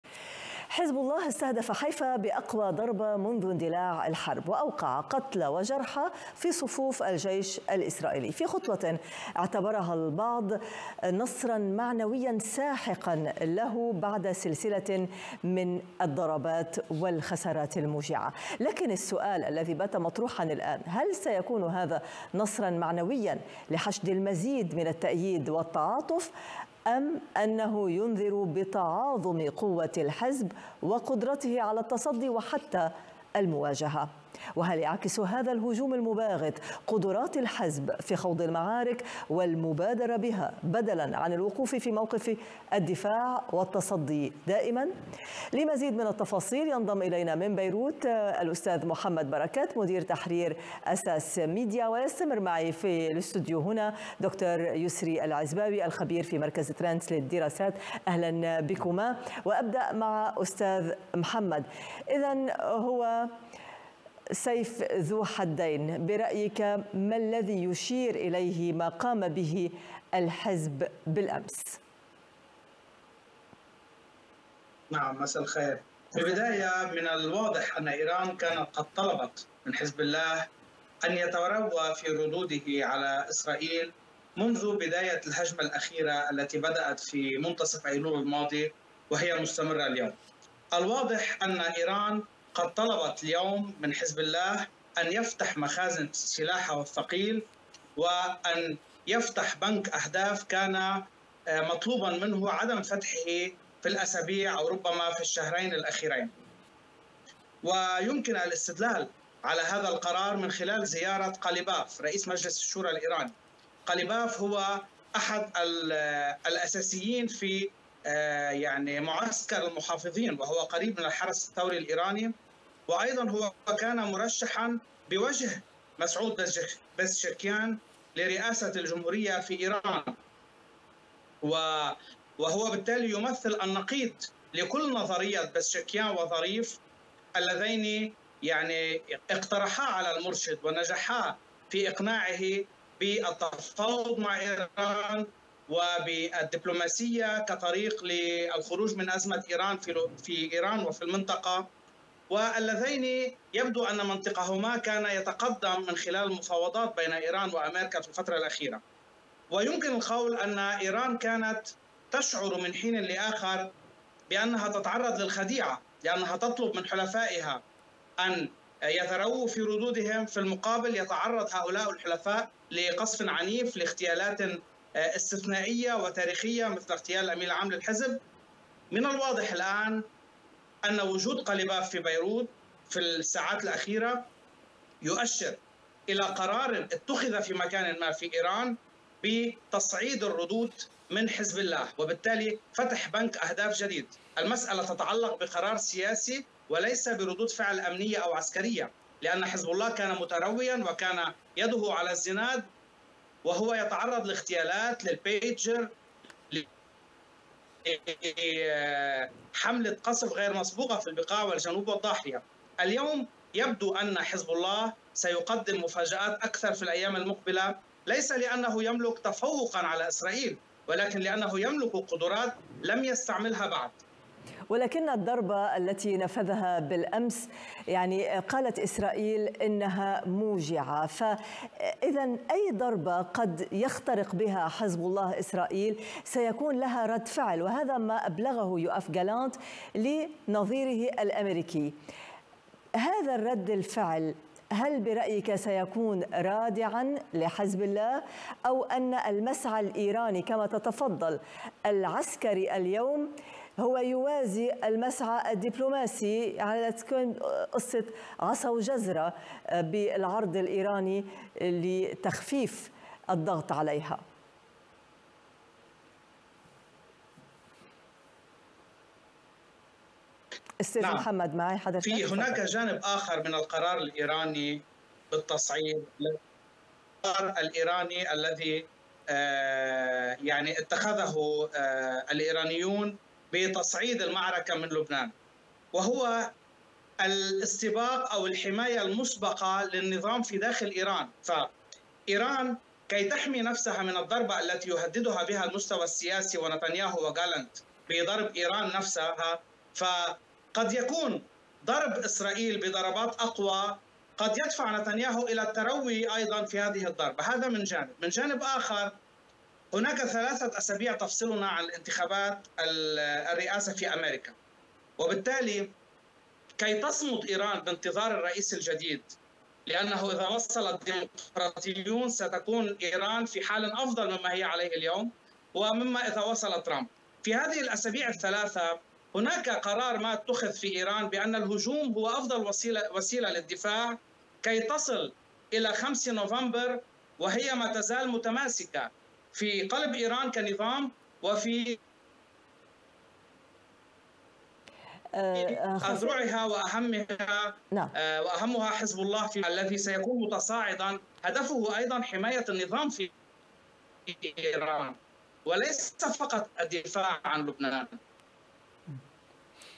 إيران منحت "حزب الله" تفويضًا بتصعيد هجماته وتوسيع بنك الأهداف
في هذه الفقرة من التغطية الخاصة "المواجهة المفتوحة"، تستضيف جمانة النونو، مدير تحرير "أساس ميديا" محمد بركات، للحديث عن التصعيد بين إيران وإسرائيل.